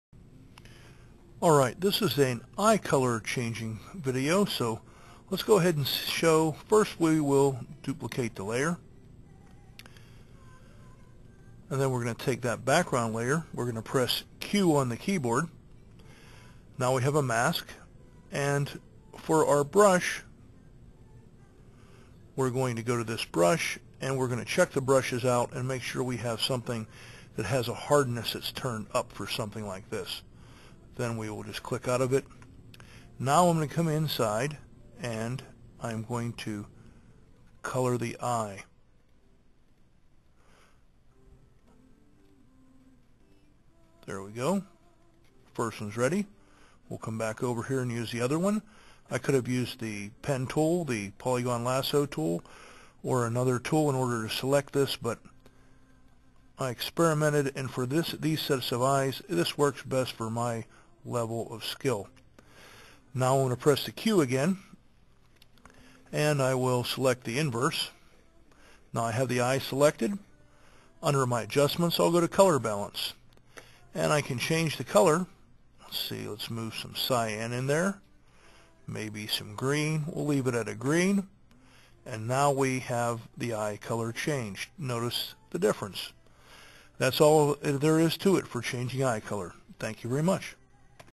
Changing Eye Color in Photoshop
Another short tutorial to show how to change Eye color